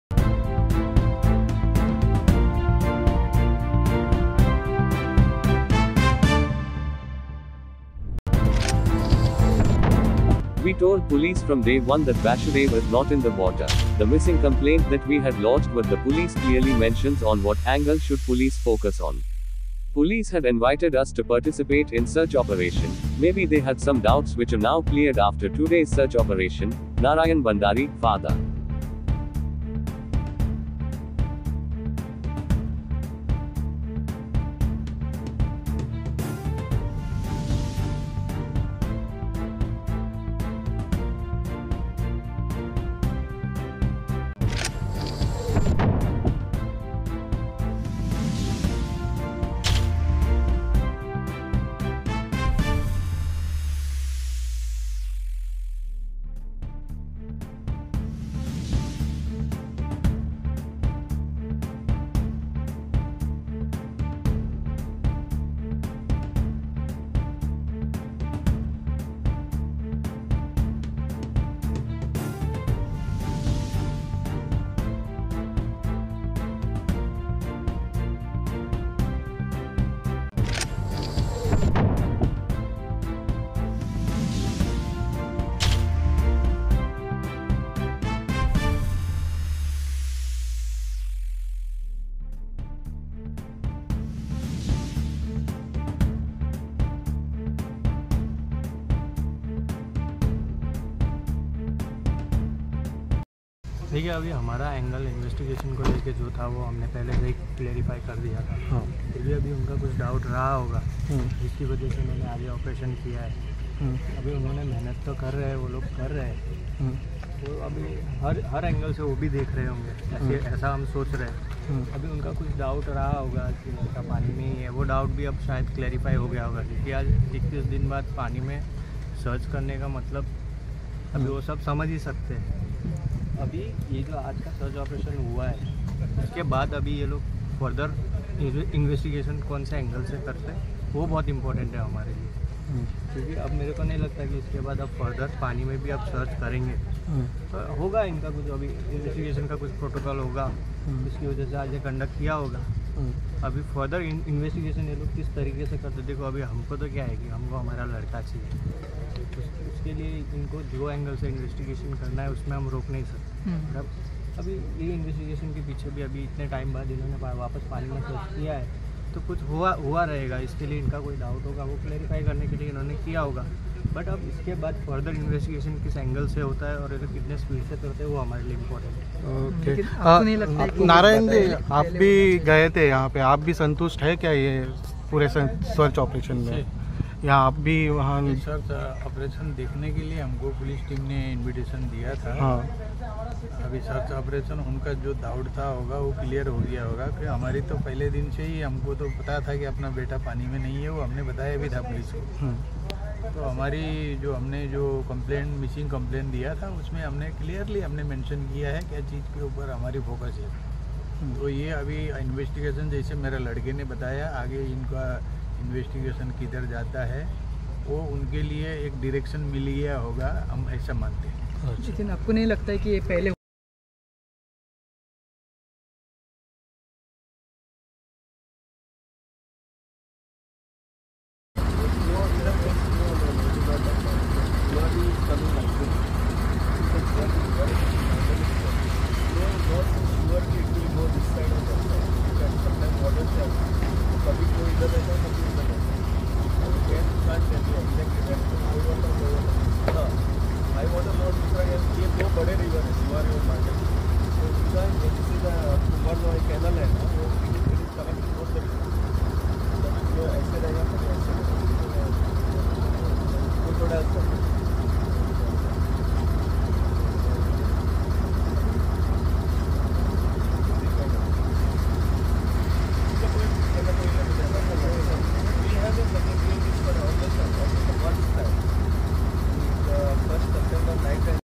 We told police from day one that Bashudev is not in the water.Bashudev Father
We told police from day 1 that Bashudev is not in the water, the missing complaint that we had lodged with the police clearly mentions on what angle should police focus on. Police had invited us to participate in search operation, may be they had some doubts which are now cleared after today's search operation: Narayan Bhandari (Father)